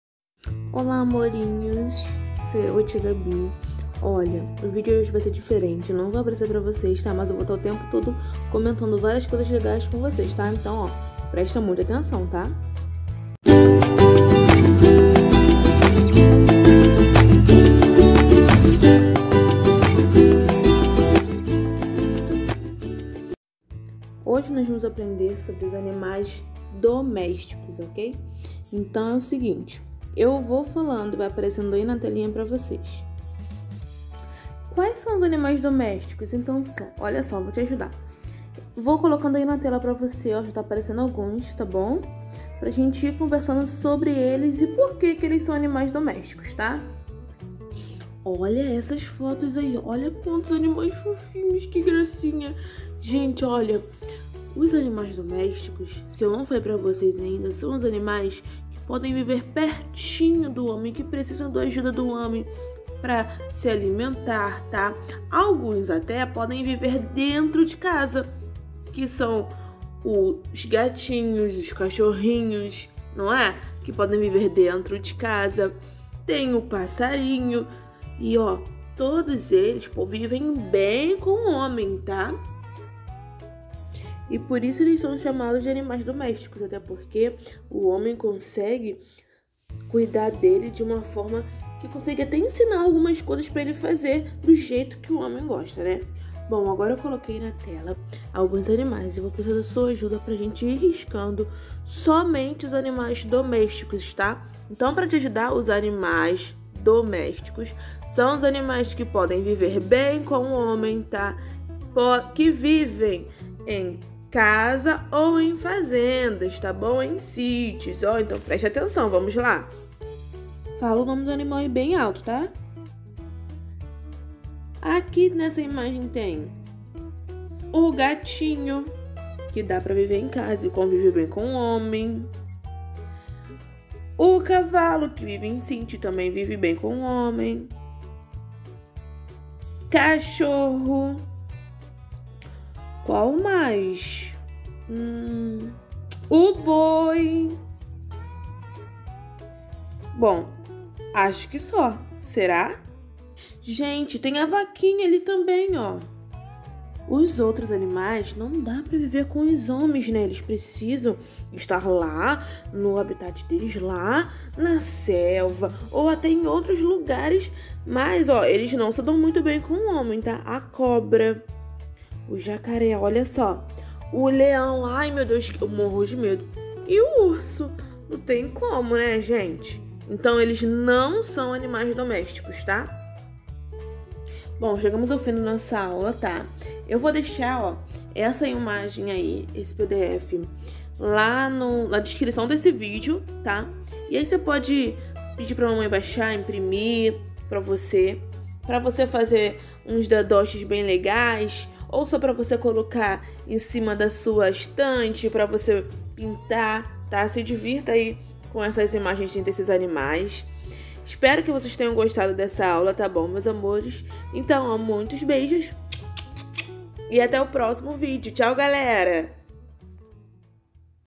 Animais domésticos - Tia Gaby
Aprendendo sobre os animais domésticos de um jeito diferente e legal!